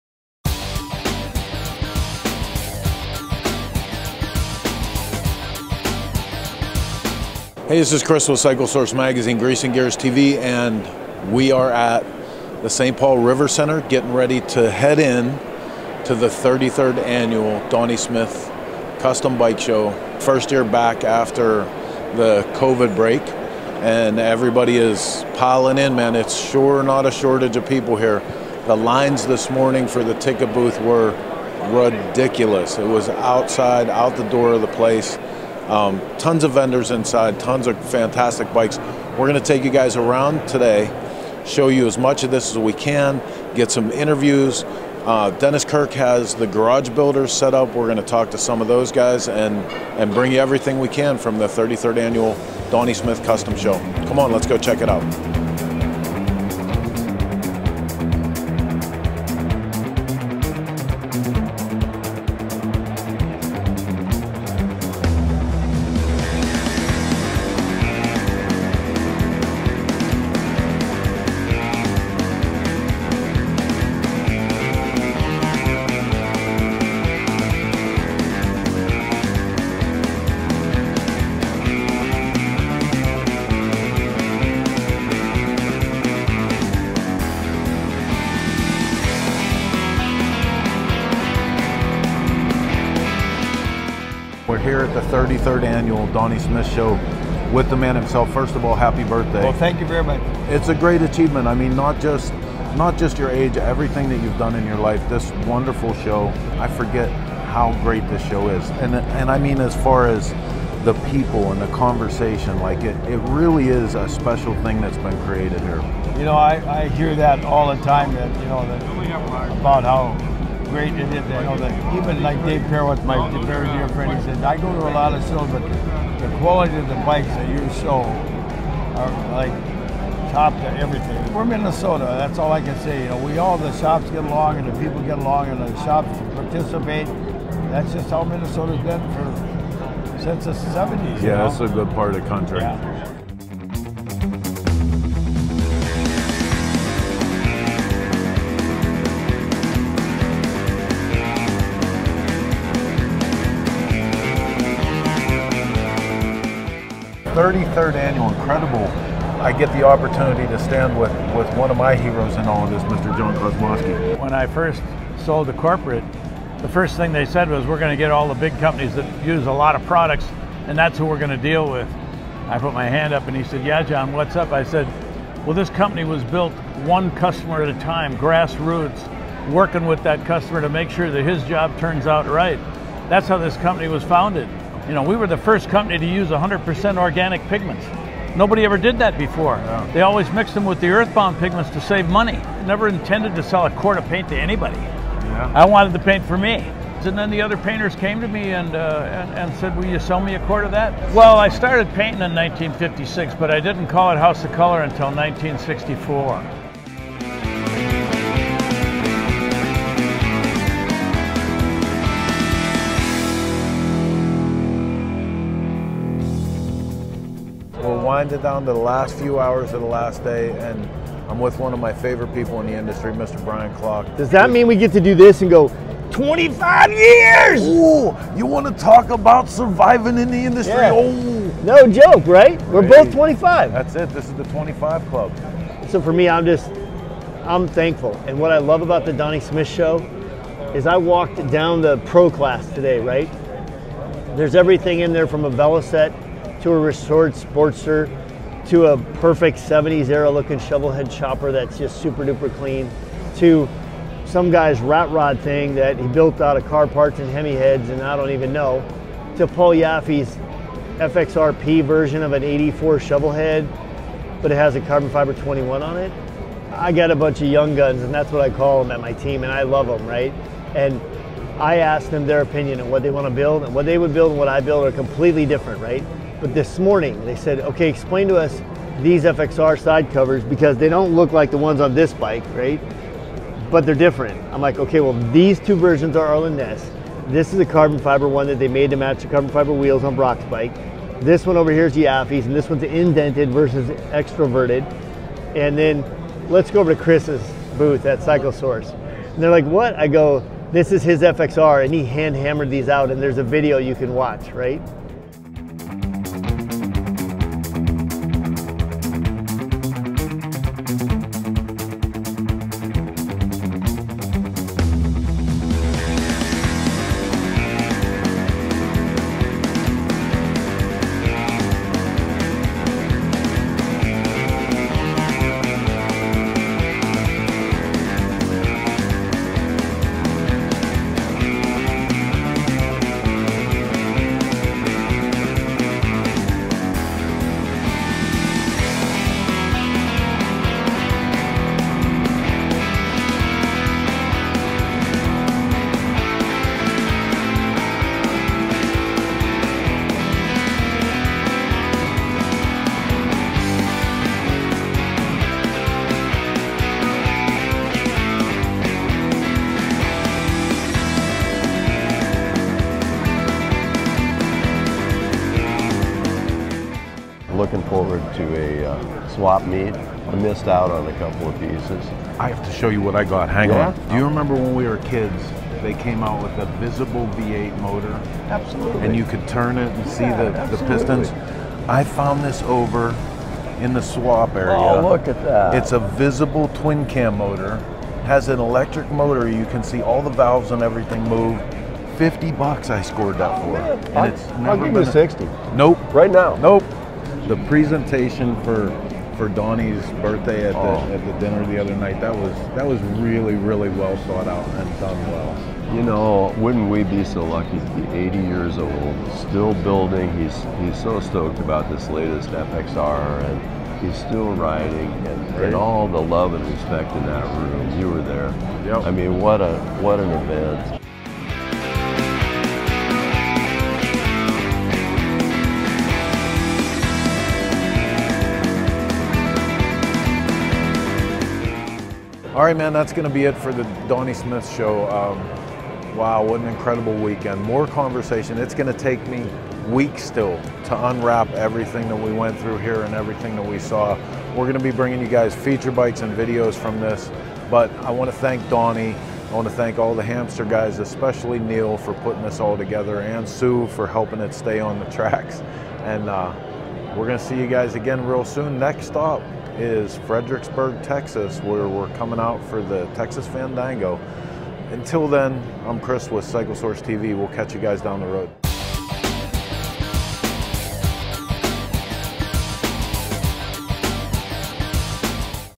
33rd Annual Donnie Smith Show 2022
The 33rd Annual Donnie Smith Show Coverage, and hey, it was Donnie's 80th birthday so an epic weekend in St. Paul!